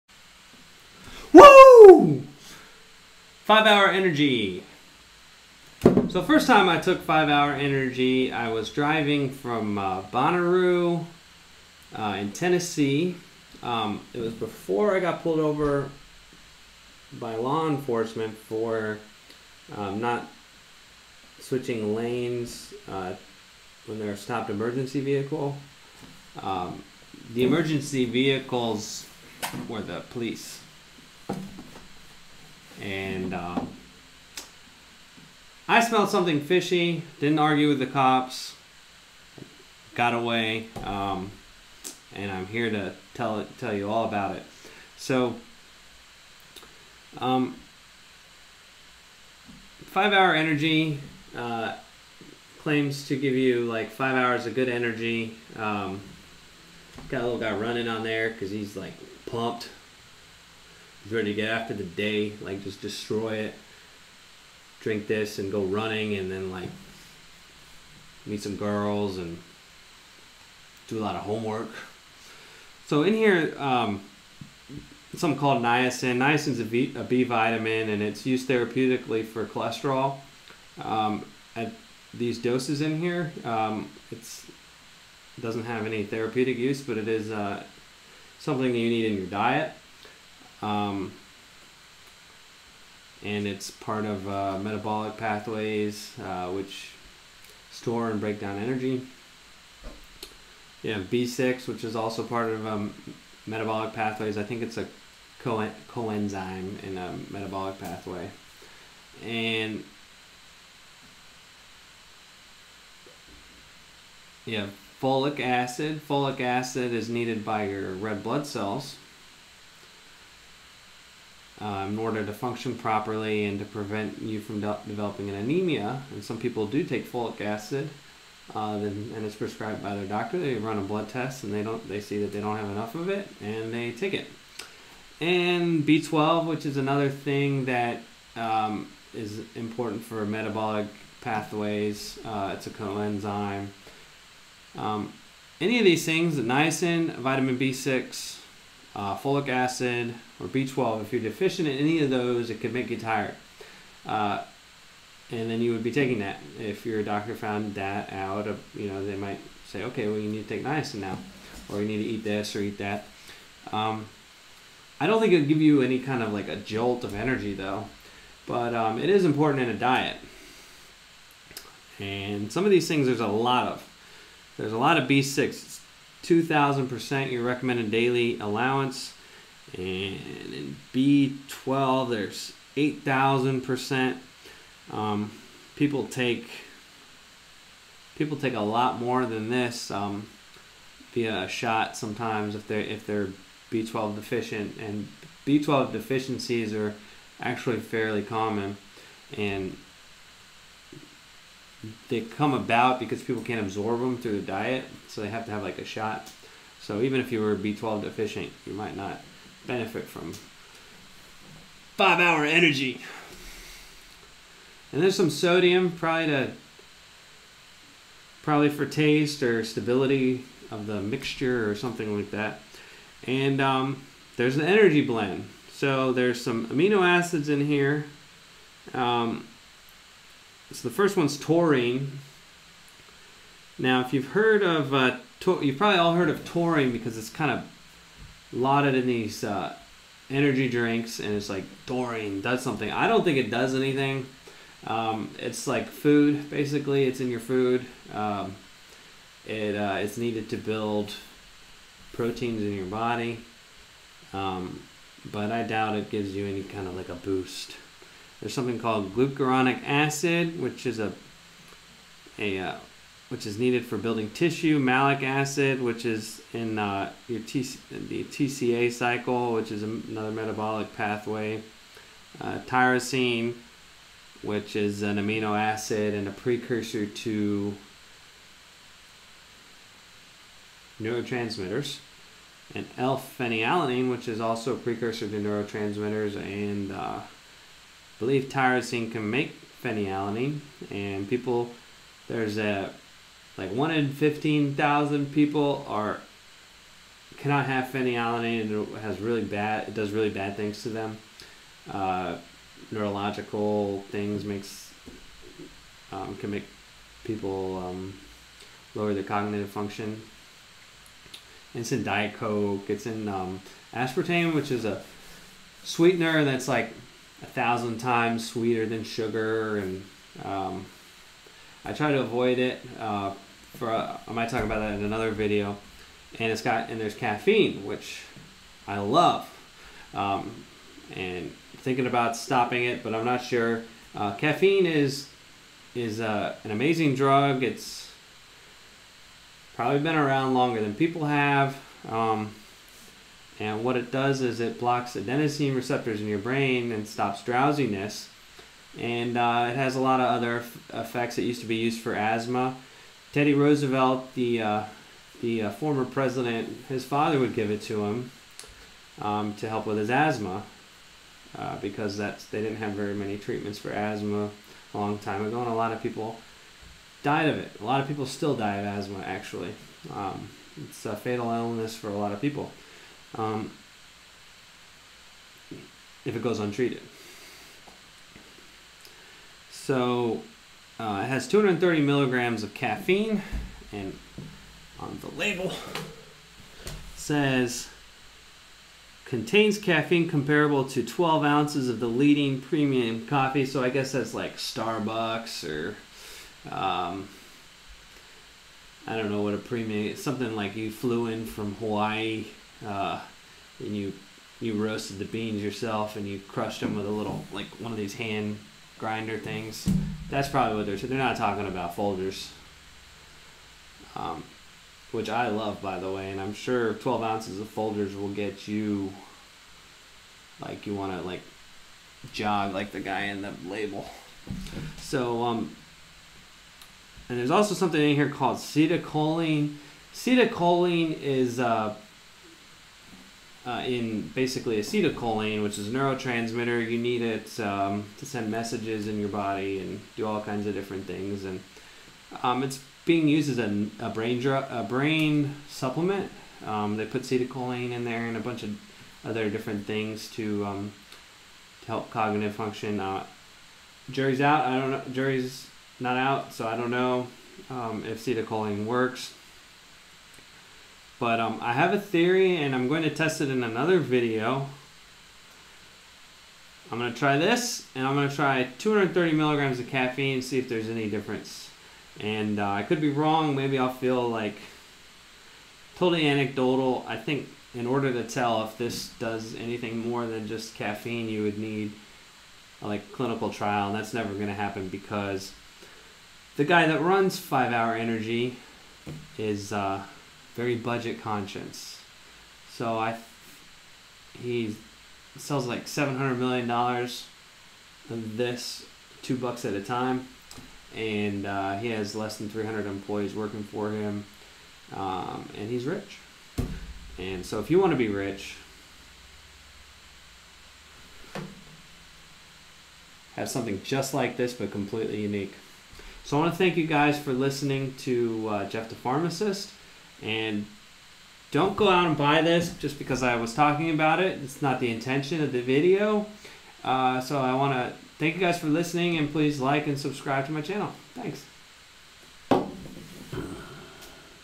5 Hour Energy Ingredients Explained
I analyze the ingredients of 5 hour energy and give my thoughts.  Does 5 hour energy do anything different than just give you a caffeine boost?